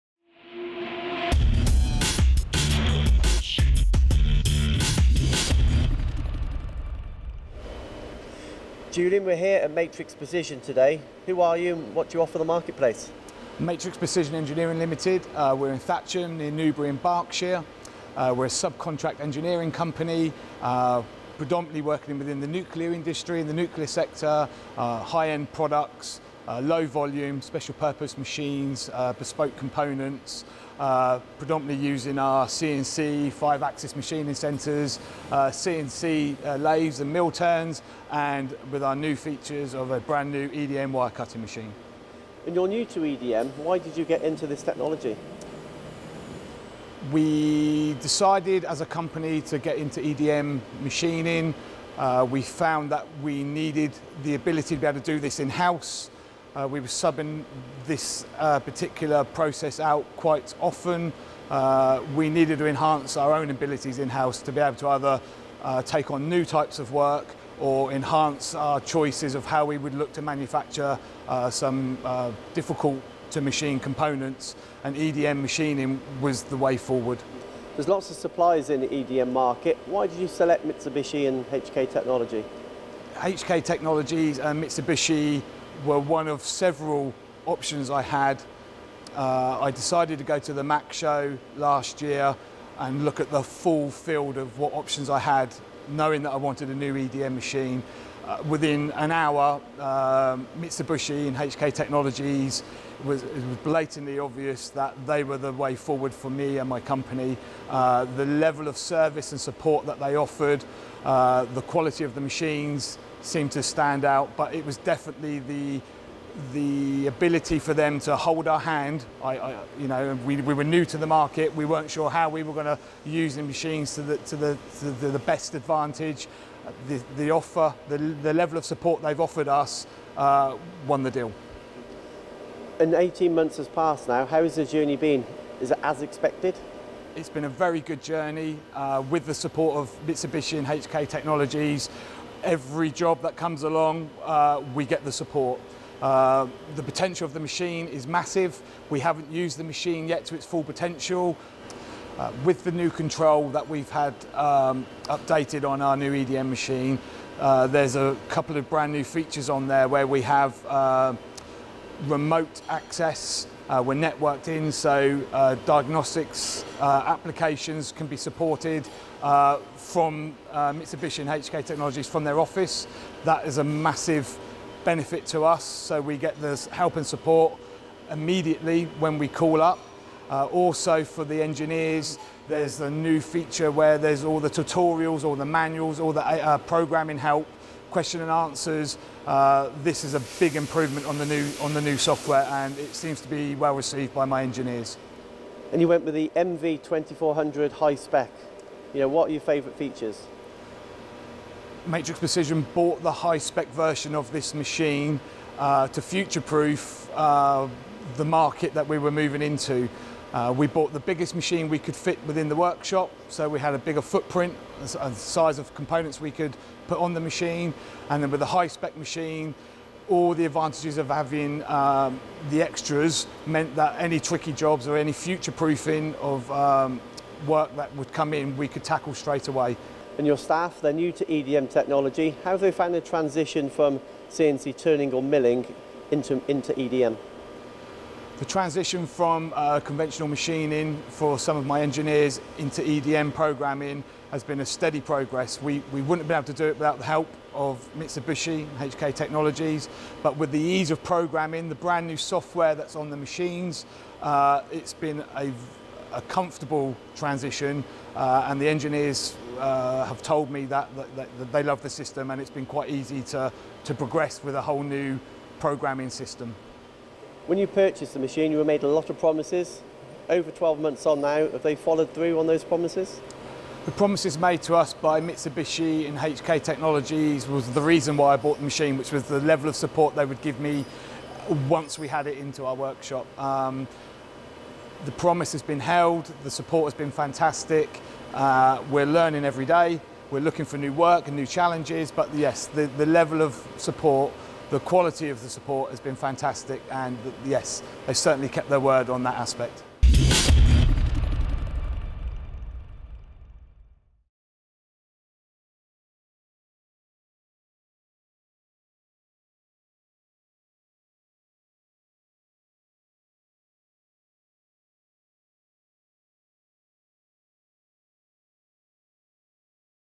Mitsubishi EDM and HK Technologies are selected by Matrix Precision
In certain circumstances traditional machining methods struggle to meet the demands of highly complex components or difficult to machine material grades.

For this reason, Matrix Precision invested in a Mitsubishi EDM Wire machine from HK Technologies. The cutting capabilities (supported by the latest software & Technology) allow them to support their customer’s ever-growing needs for complex & difficult to machine parts.

EDM Wire-Cut parts are extremely precise & accurate on any grade of electrically conductive materials. The EDM process creates electrical sparks between the component and the Brass wire of the EDM machine (Brass wire size is just 0.25 mm in diameter). The CNC programmed path of the wire will move along the profile form to be cut & the sparks created remove the material to be cut away.

Watch this video to see why Mitsubishi EDM and HK were selected by Matrix.